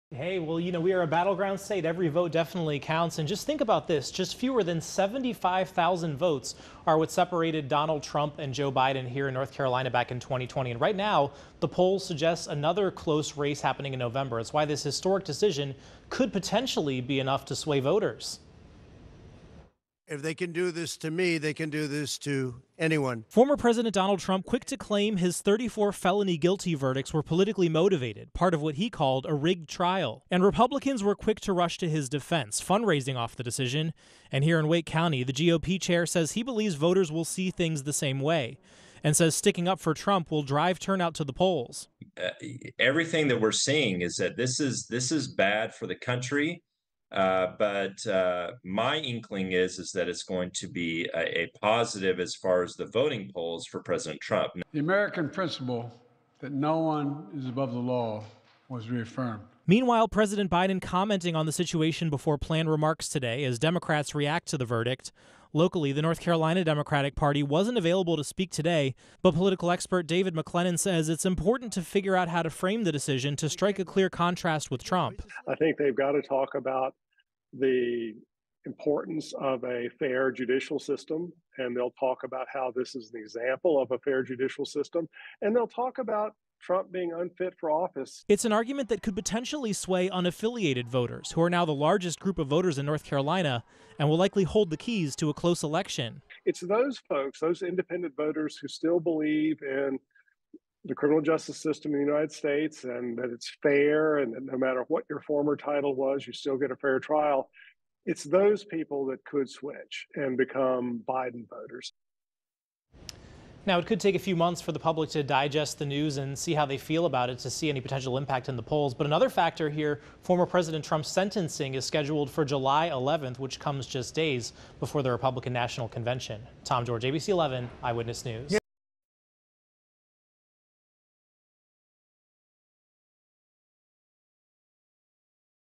Trump guilty verdict impact on race in North Carolina
In 2020, just fewer than 75,000 votes separated Donald Trump and Joe Biden in North Carolina. Right now, the polls suggest another close race in November.